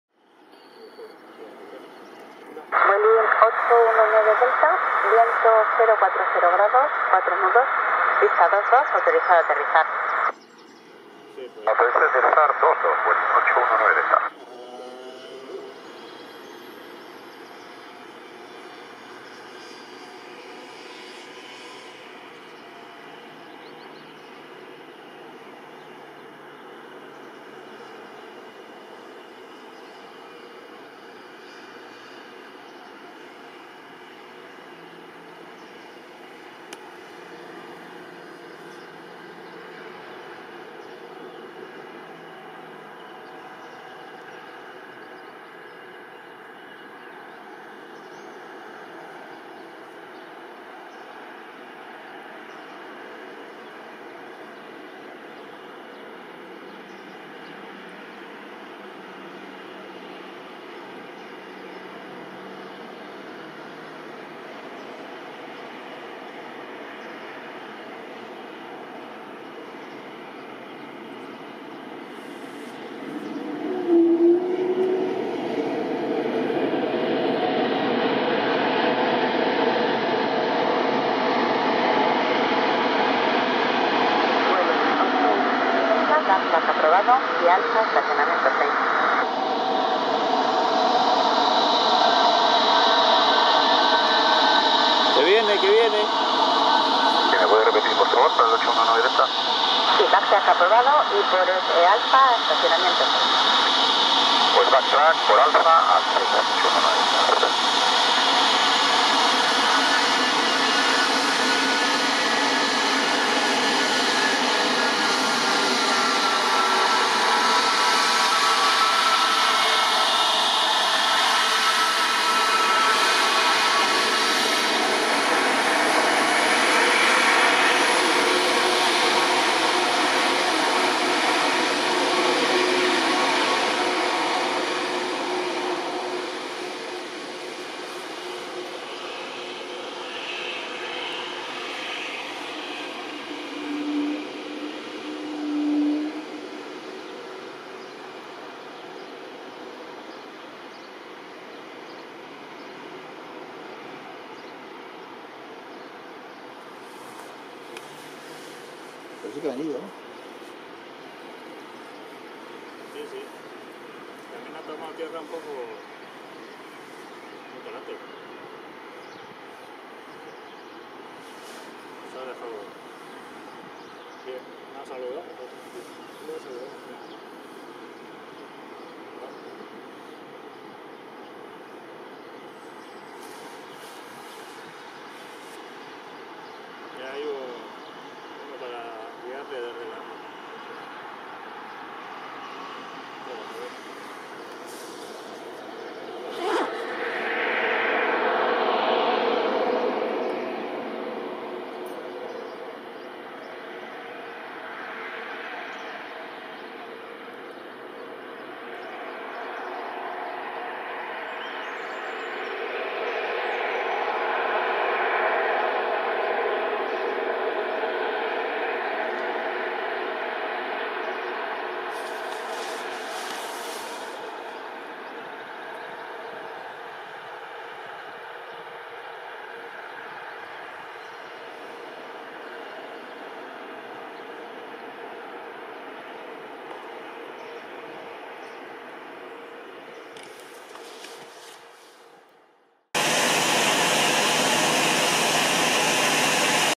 4K Airbus A320 NEO, EC-NAX Aterrizando pista 22 Aeropuerto San Sebastián
4K Airbus A320 NEO,  EC-NAX Aterrizando pista 22 Aeropuerto San Sebastián.
No es lo normal que este avión de la compañía Vueling opere en San sebastián. "IMPRESIONA" ver los dos motores que tiene este modelo NEO.